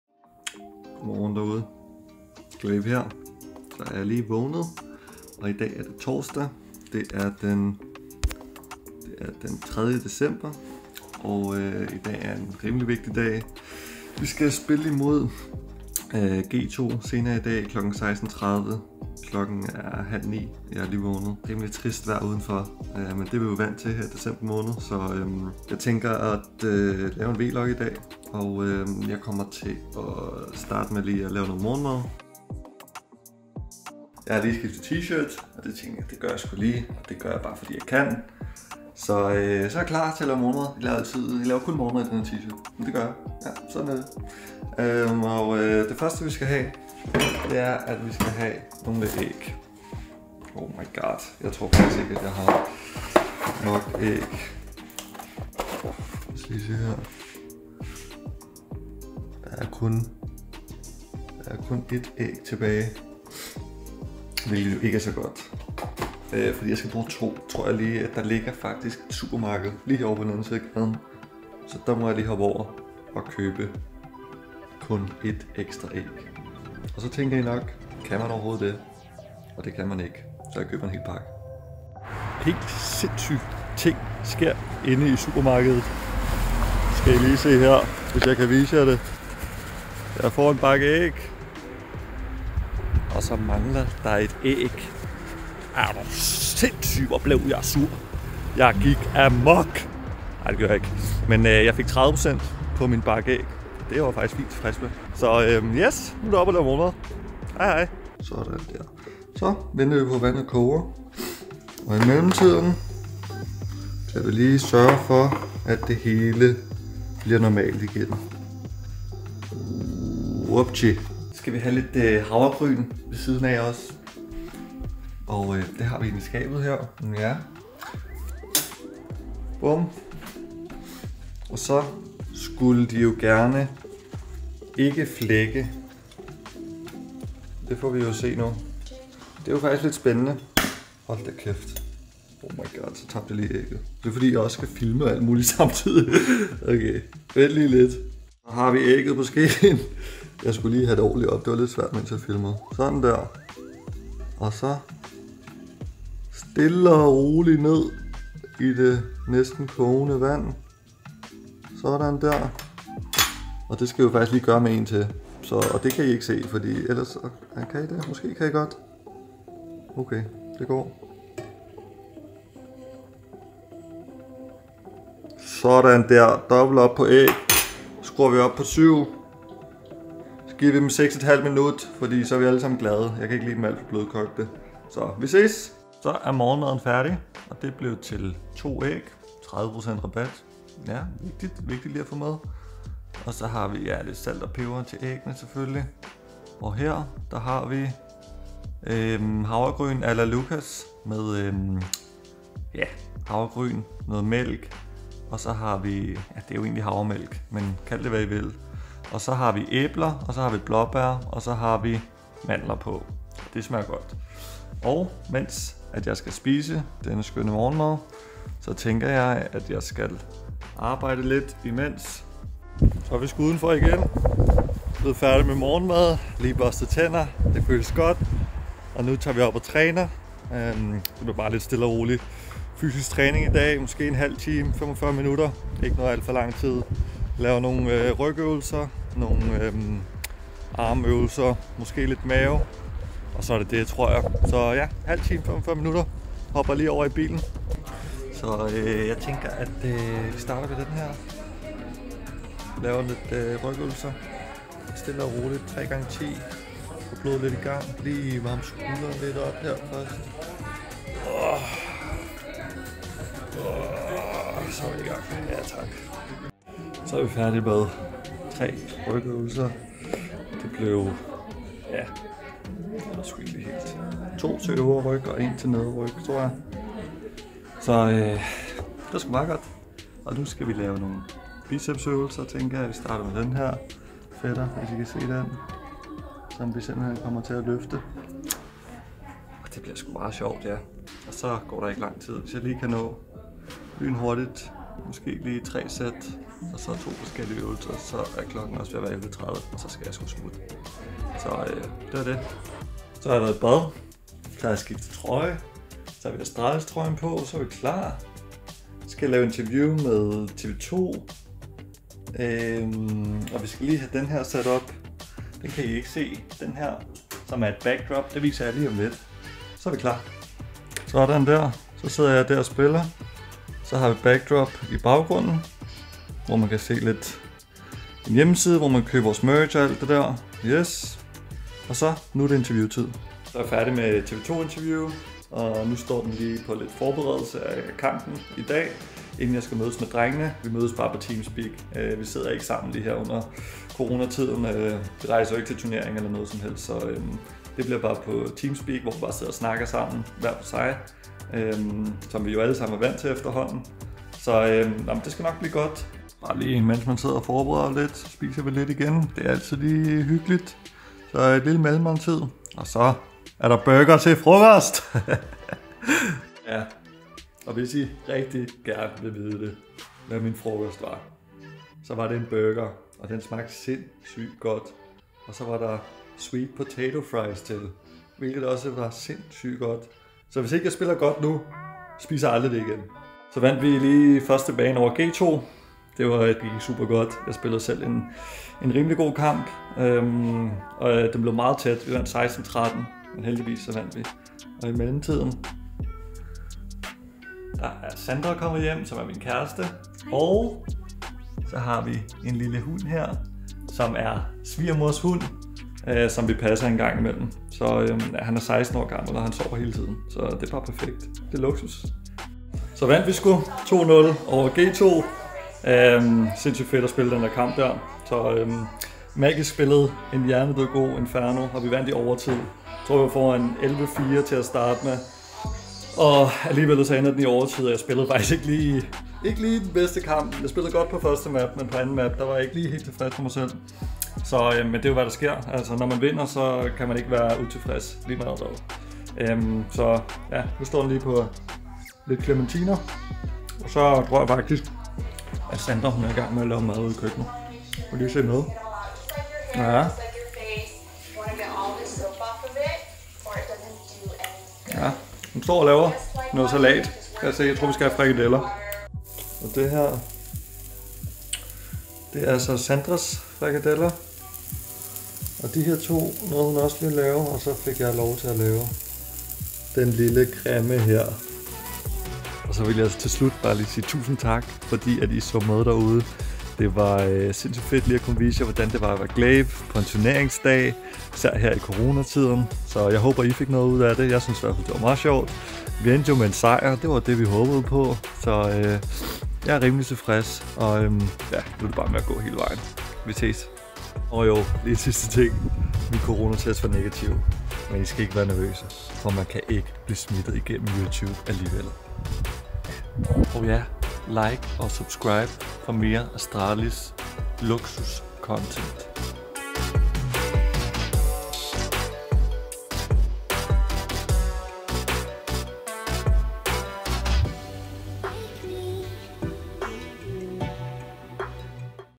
gla1ve til gameday... og i køkkenet! | Vlogs med Astralis
gla1ve tager jer med helt tæt på til en dag med mad, træning, CS, mad og mere mad.